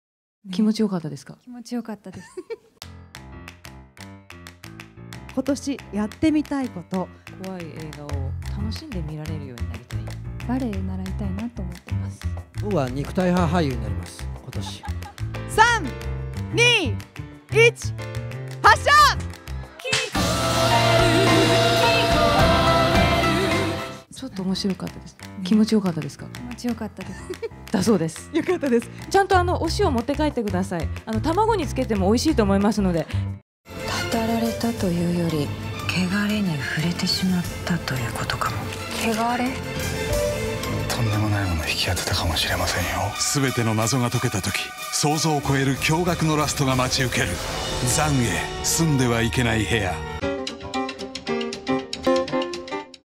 竹内結子&橋本愛 バズーカで気持ちいい！？
小野不由美氏の小説が原作の映画「残穢−住んではいけない部屋−」の完成披露試写会が行われ、
主演の竹内結子さん、橋本愛さん、佐々木蔵之介さんらが登壇しました。

年の始めのイベントと言う事で、「今年克服したいこと・挑戦したいこと」を問われると、
竹内結子さんは「怖い映画をちゃんと楽しんで見られるようになりたい」と、
今回の作品が怖すぎて試写が出来なかった事を振り返りました。

観客には、お清め塩を出演者一同からバズーカでお届け。
橋本愛さんは「気持ちよかった」とご満悦の様子でした。

映画「残穢−住んではいけない部屋−」
2016年1月30日　全国公開